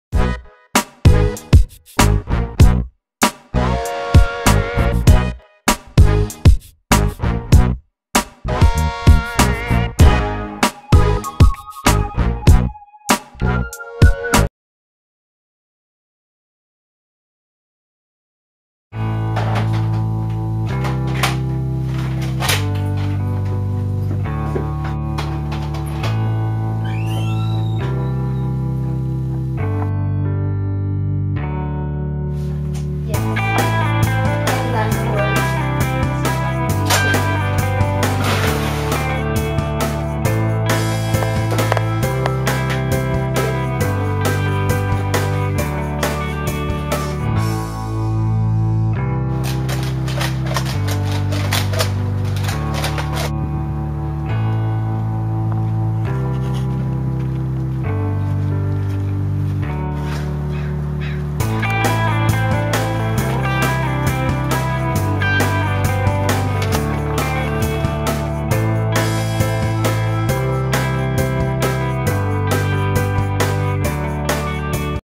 Nerf war: Betrayal trailer
My first Nerf war, the actual thing should be out before Friday.